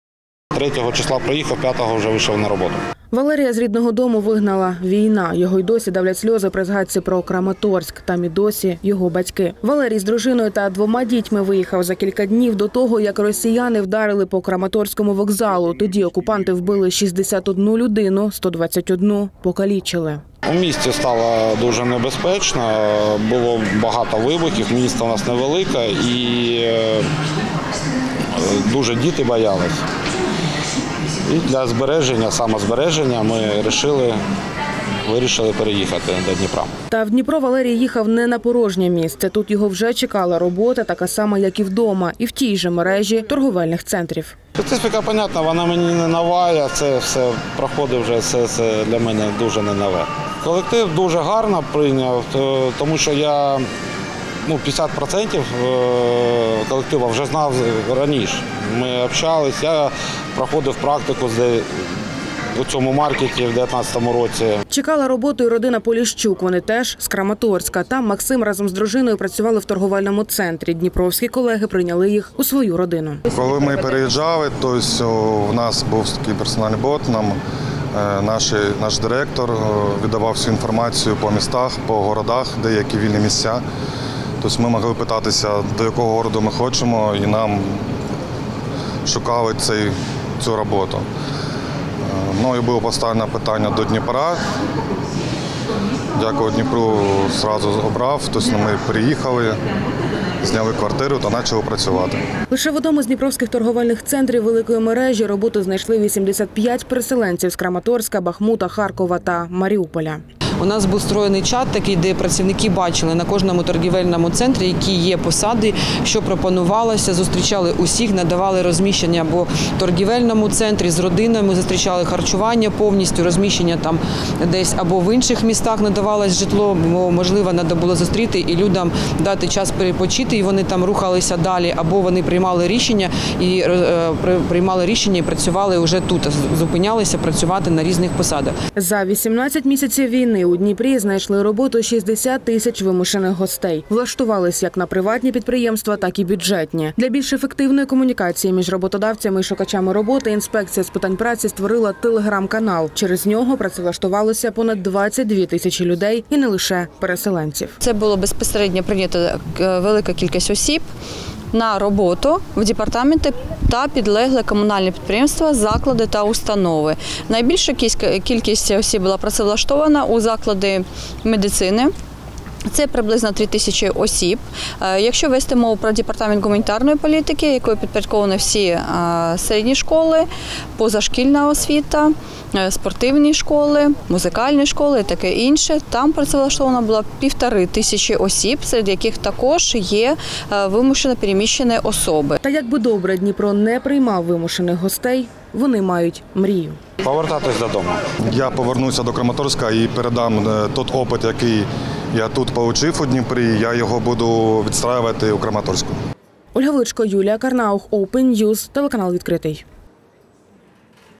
Понад півмільйона людей офіційно працевлаштовані у Дніпрі
Понад півмільйона людей офіційно працевлаштовані у Дніпрі.  За час повномасштабної війни кількість працюючих не впала, а навпаки значно зросла -  на понад 150 тисяч. Робочі місця пропонує і бізнес, і бюджетні організації. Особливий вектор уваги на переселенців. Та чи задоволені вони  на новому місці?